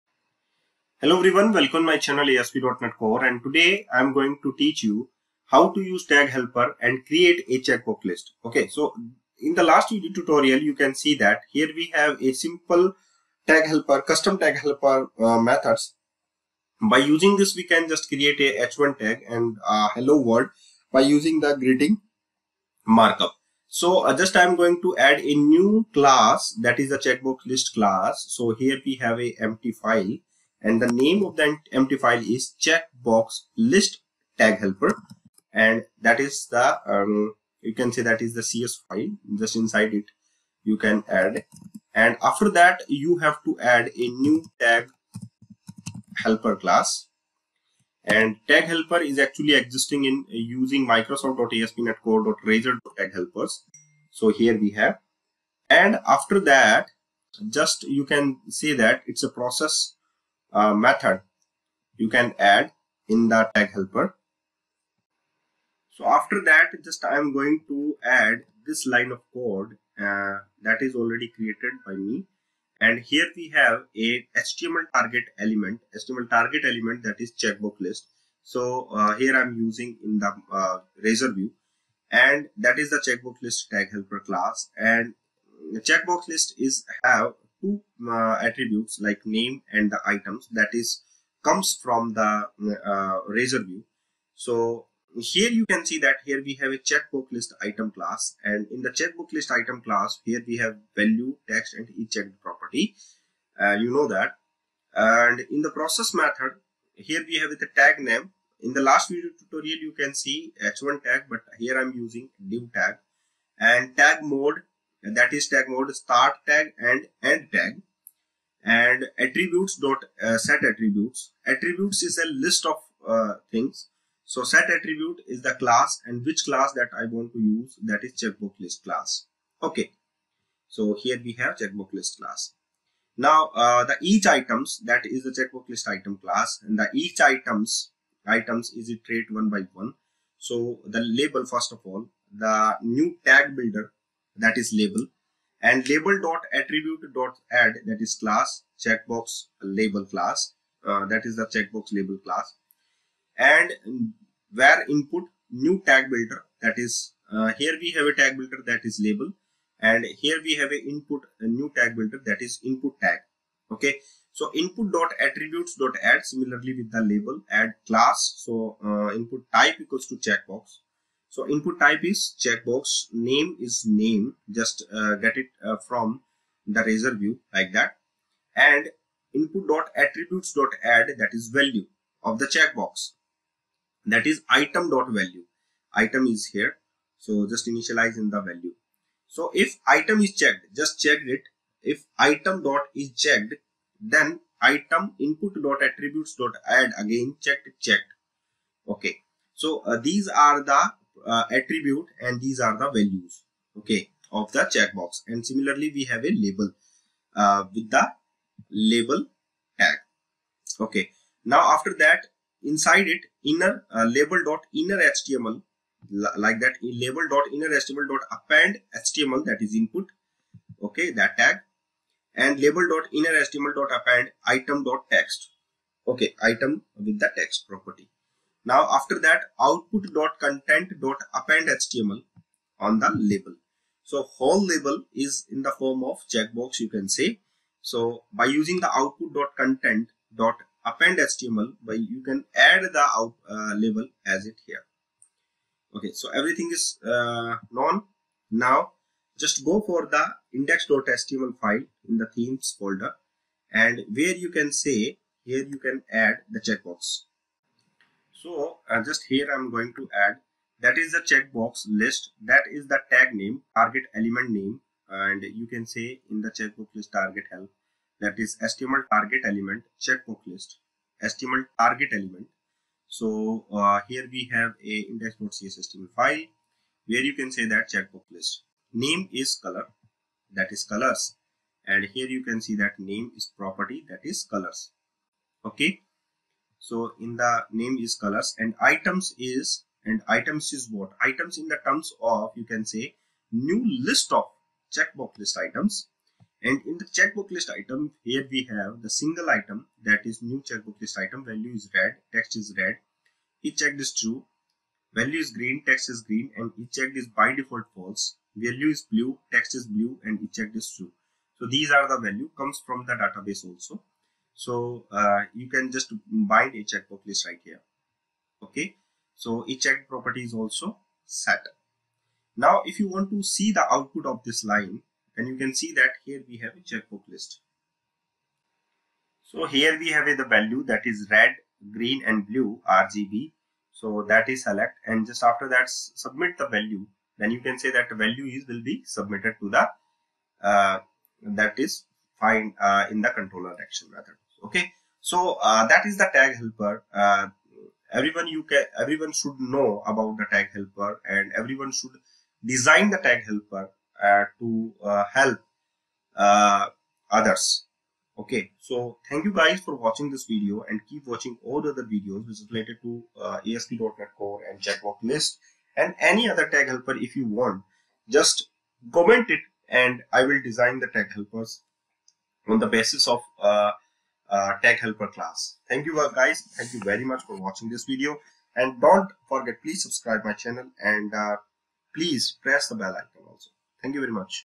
ASP.NET CORE Checkbox List Tag Helper Bind with Data
In this video, I am going to teach you, create a CheckBox List Tag helper which is bind with data.